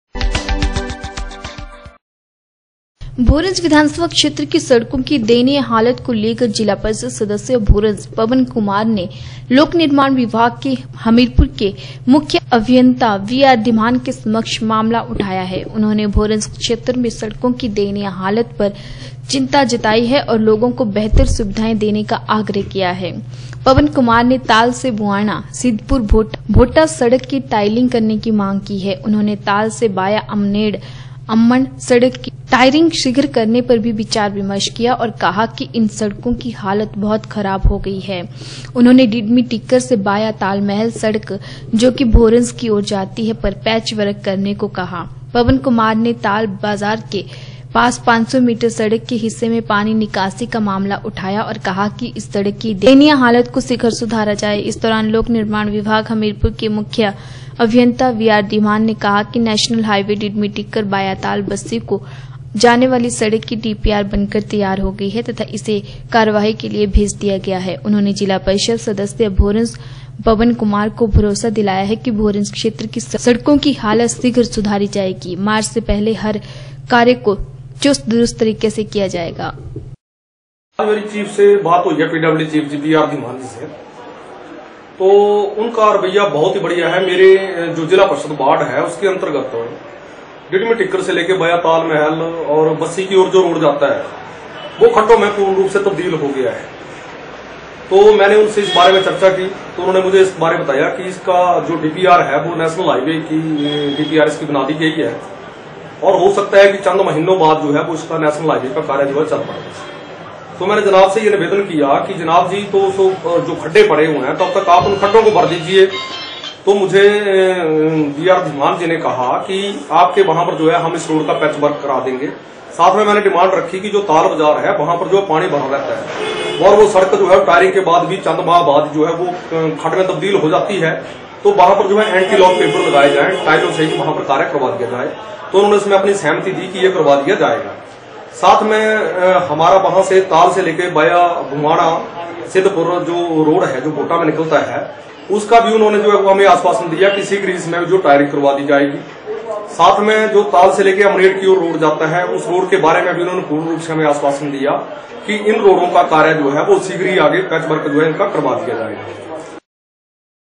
पवन कुमार ने लोक निर्माण विभाग के मुख्य अभियंता बीरआर धीमान के समक्ष मामला उठाया है
भोरंज विधानसभा क्षेत्र की सड़कों की दयनीय हालत को लेकर जिला परिषद सदस्य भोरंज पवन कुमार ने शनिवार को लोक निर्माण विभाग हमीरपुर के मुख्य अभियंता बीरआर धीमान के समक्ष मामला उठाया है। उन्होंने भोरंज क्षेत्र में सड़कों की दयनीय हालत पर ¨चता जताई हैं और लोगों को बेहतर सुविधा देने का आग्रह किया है।